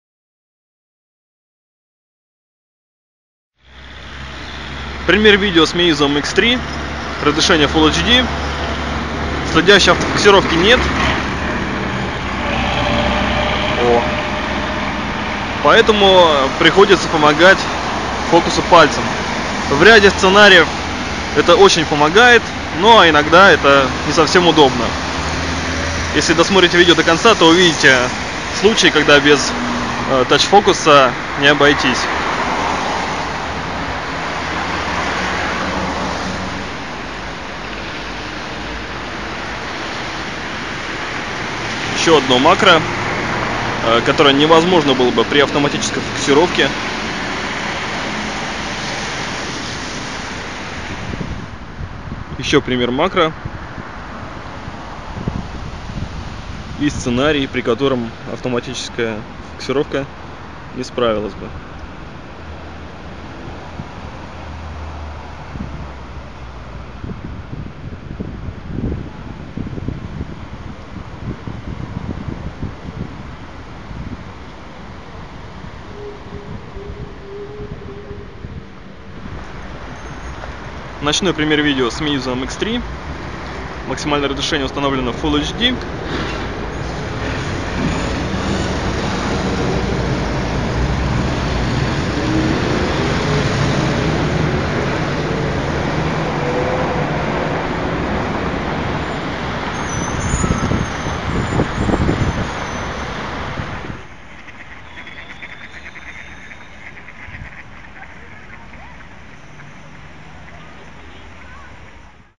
Камера Meizu MX3: примеры видео в разных условиях (camera test, sample)
Камера Meizu MX3: примеры видео в разных условиях (camera test, sample). Съемка макро и издалека, демонстрация тач-фокуса, стабилизация, запись звука.
Video: AVC at 50-80 Mbps
Aspect: 1920 x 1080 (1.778) at 34.677 fps
Audio: AAC at 96.0 Kbps
Infos: 1 channel, 16.0 KHz